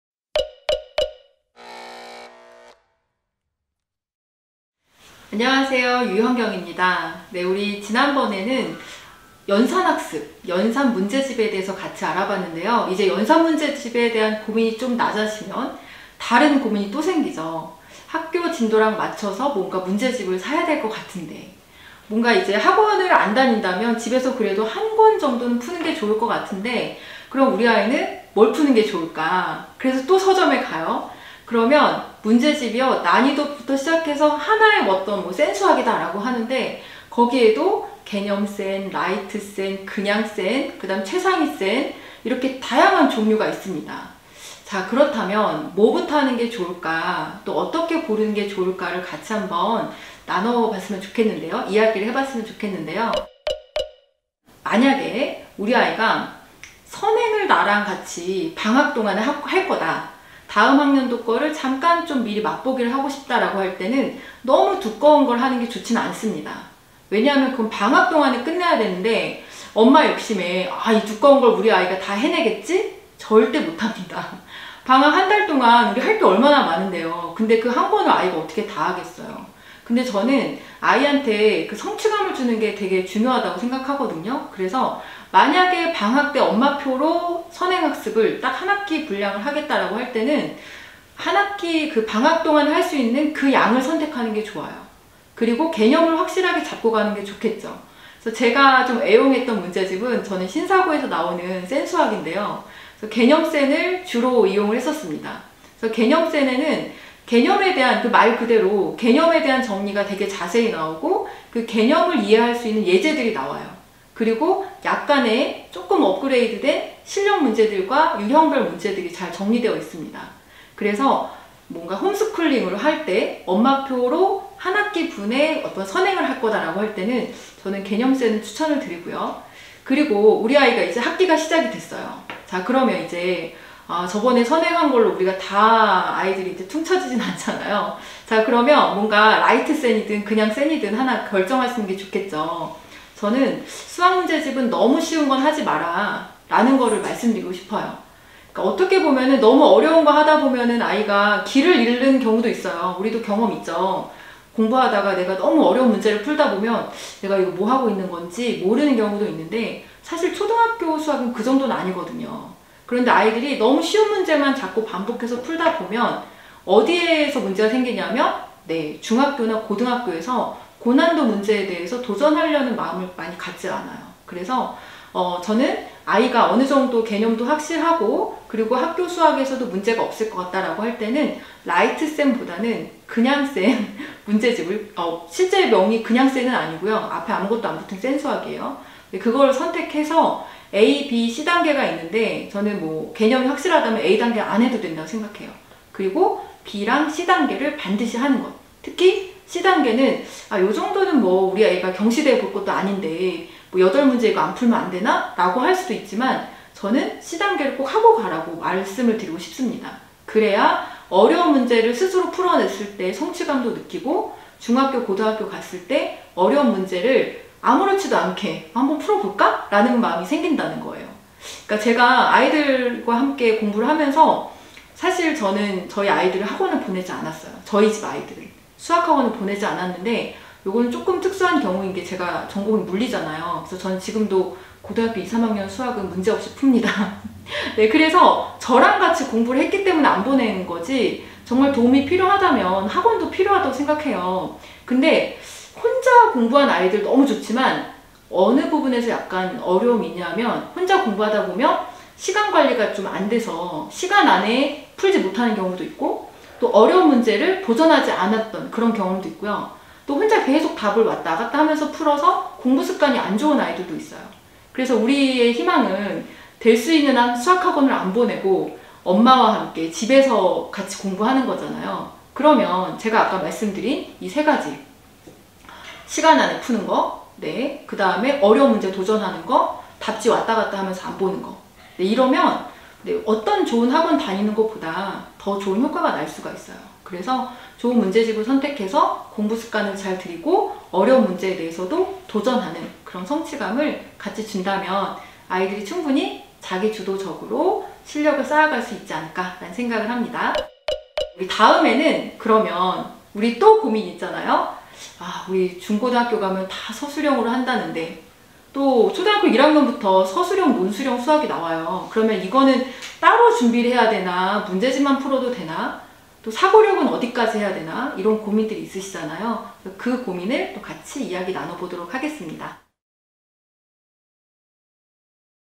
[갈수록 성적이 오르는 초등공부법] #2.수학문제집 선택 방법
초등, 중등, 고등 세 아이의 엄마이자 / 21년동안 중, 고등교사로 일하고 / 단국대학교 창업대학원 겸임교수를 맡은 / 육아의 달인, 유현경입니다.
중학생, 고등학생이 될수록 공부를 더 잘하는 아이가 되려면 어떻게 해야 할까요?
그 두 번째 시간, 아이에게 맞는 수학문제집을 선택하는 방법을 알아봅시다.

소울tv 소울하우스 갈수록성적이오르는초등공부법 - 매주 화, 목요일 업데이트됩니다